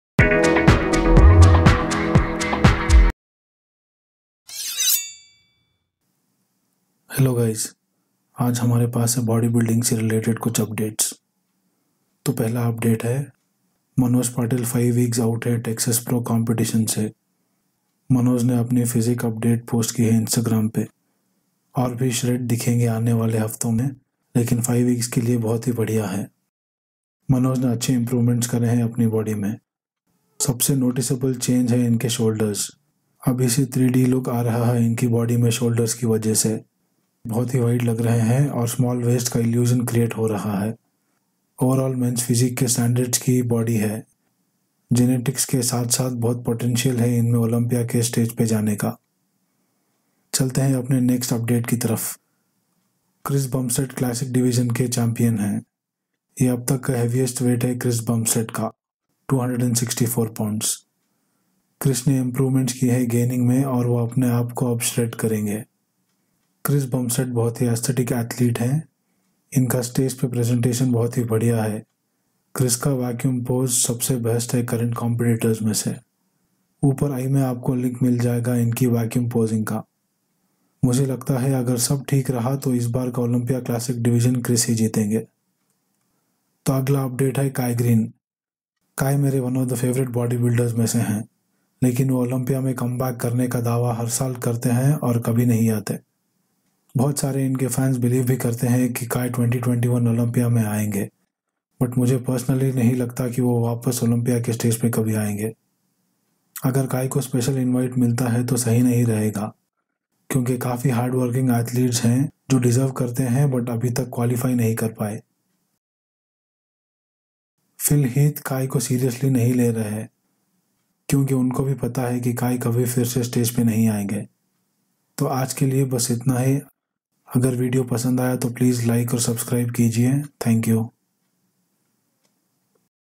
Manoj Patil 5 wks out- Texas Pro| Chris Bumstead heaviest ever| Phil Heath making fun of Kai Greene?
This video includes updates from the world of bodybuilding.
Manoj Patil posting an update 5 wks out of Texas Pro, Chris Bumstead heaviest ever, Phil Heath making fun of Kai Greene.